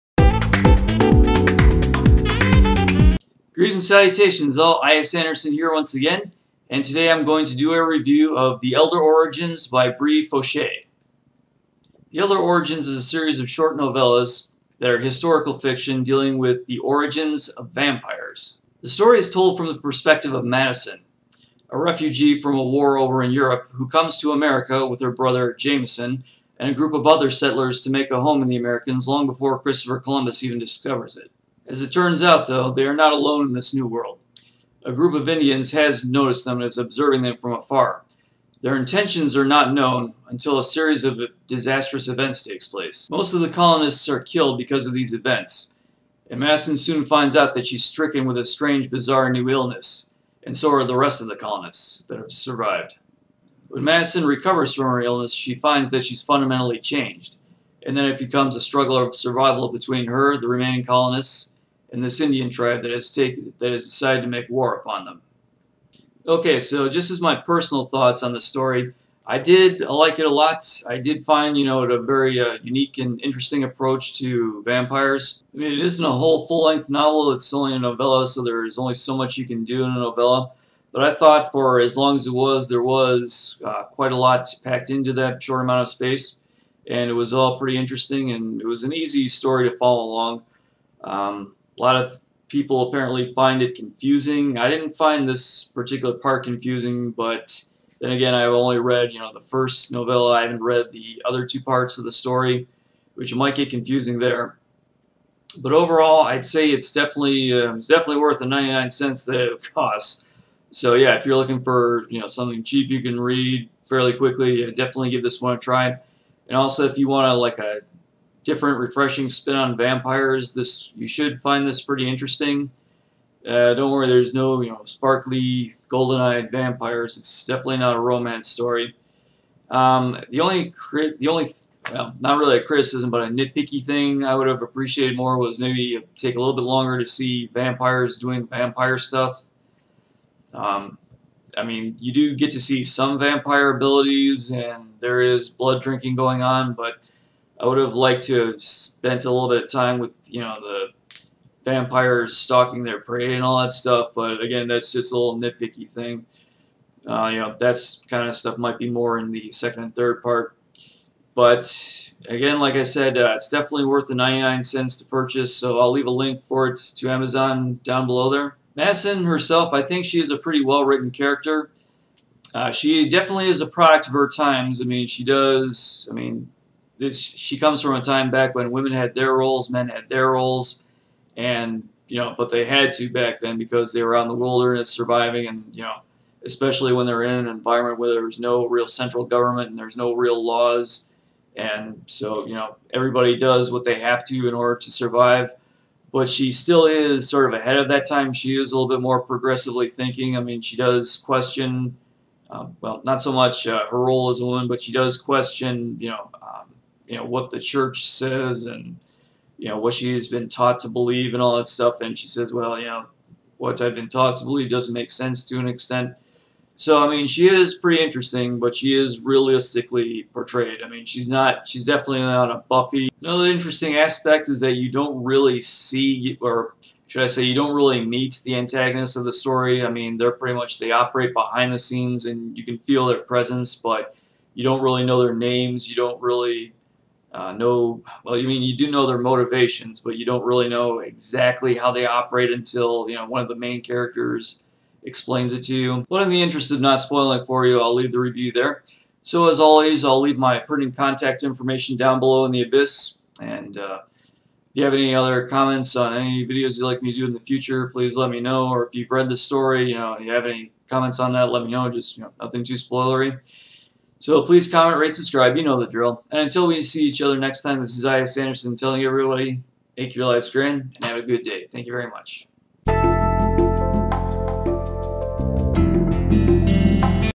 Book Review - The Elder Origins by Bre Faucheux
I review a novella about the origins of vampires. Don't worry, nobody sparkles here.